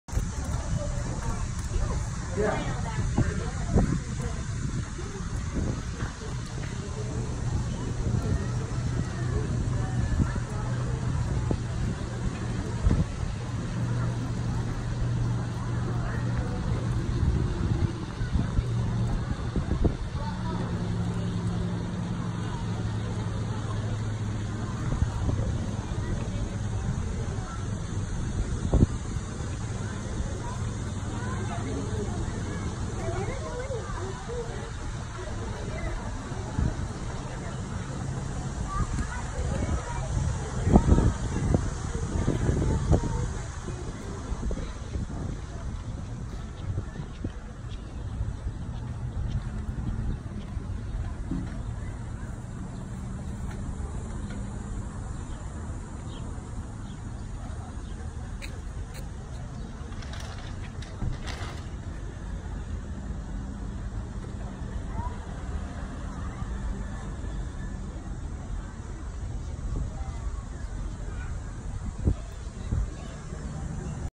St. Joe, Michigan hanging out on the bluff
Local plumber loves to visit St. Joe, Michigan with his family